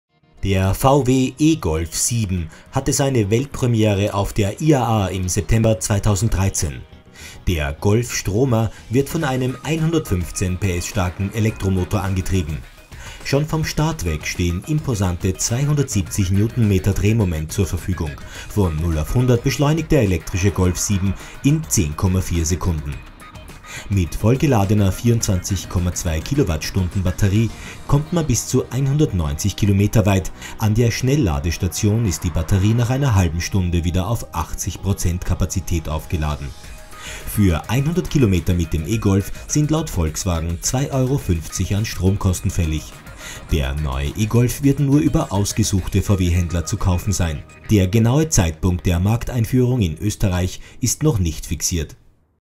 VW e-Golf 7 in Serienversion
Auf der IAA 2013 hatte der elektrische Golf 7 seine Weltpremiere. Die E-Community wartet schon ungeduldig auf die Markteinführung - hier erste Informationen im Video.